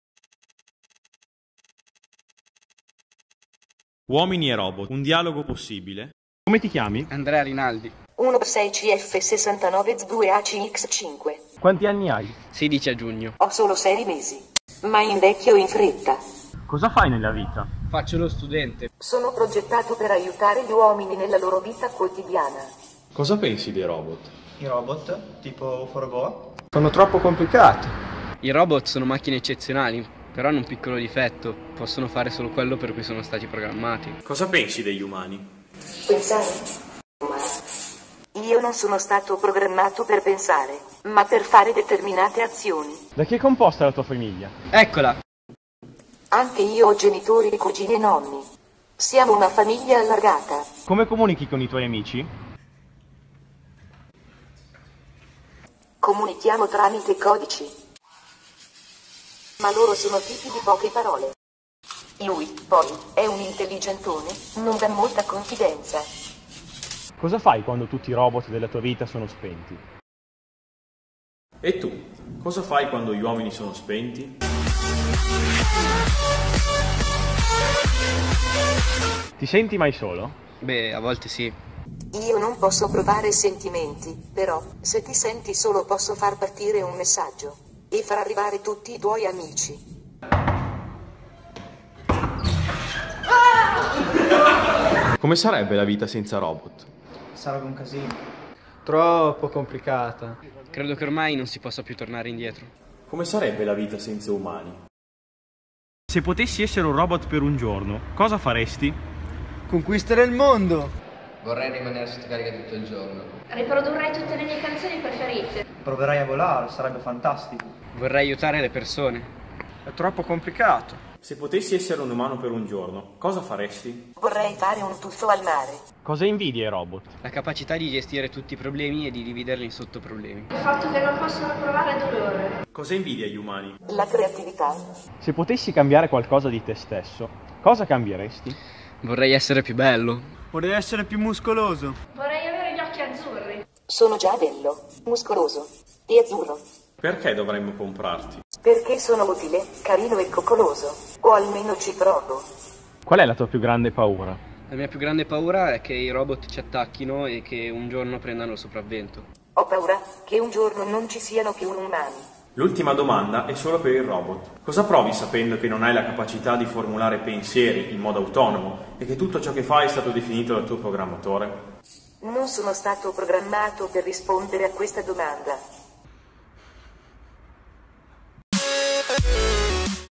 ISII Piacenza concorso "Io narRO..BOT"
Un'intervista (im)possibile. La 2F per il concorso AICA/USR Emilia Romagna 2016/2017